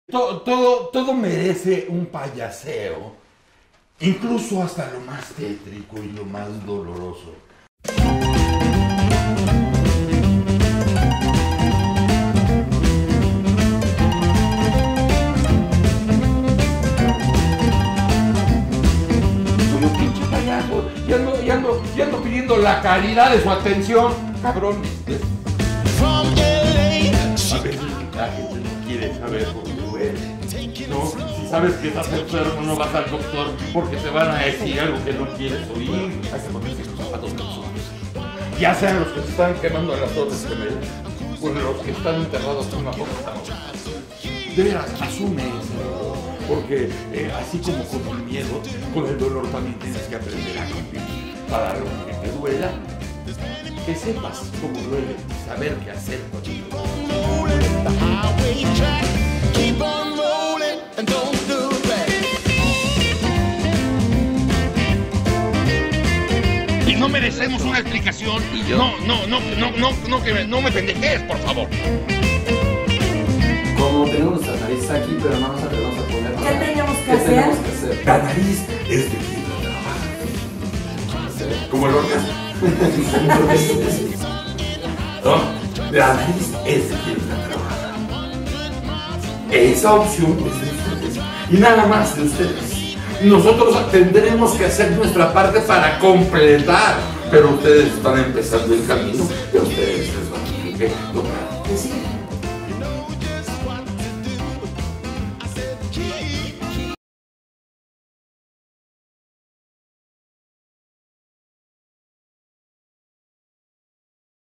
Brozo: Asume el miedo y ponte la nariz | Mapas Mentales
Entrevista con Brozo, el "payaso tenebroso" que no soporta mentiras, chantajes, ni payasadas.

Créditos

Director: Diego Bolaños/Alán Hernández/Daniel Vidaurri
Producción: Diego Bolaños/Daniel Vidaurri/Coco Uruchurtu/Patty Maldonado
Fotografía: Daniel Vidaurri
Edición: Daniel Vidaurri/Christopher Mariscal/ Isaac Salas
Investigación y entrevista: Alan Hernández/Regina Mendoza

Producción original de Cultura Colectiva.